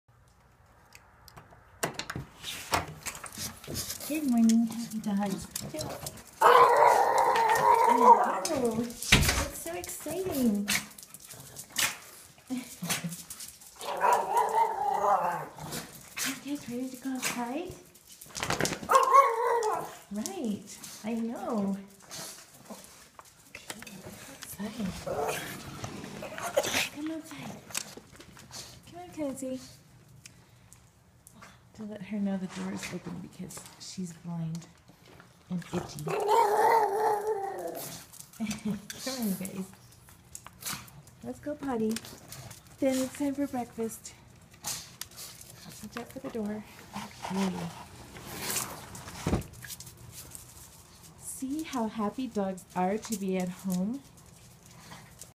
Pug Love
She is always so vocal with her glee to see the pet sitter come!